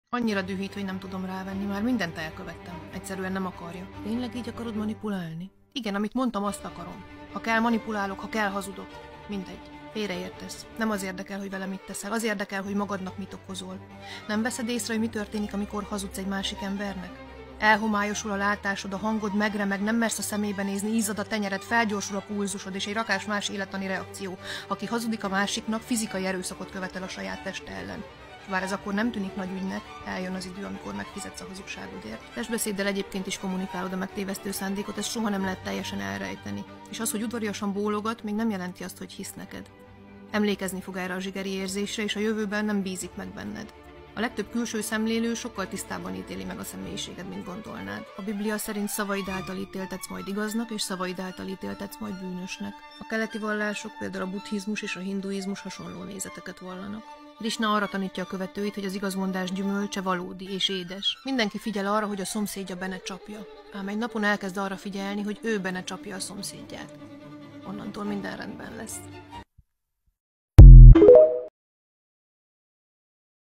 Hazugság - Manipuláció
A boldogság és az őszinteség közötti összefüggés - Lewis M. Andrews könyve alapján.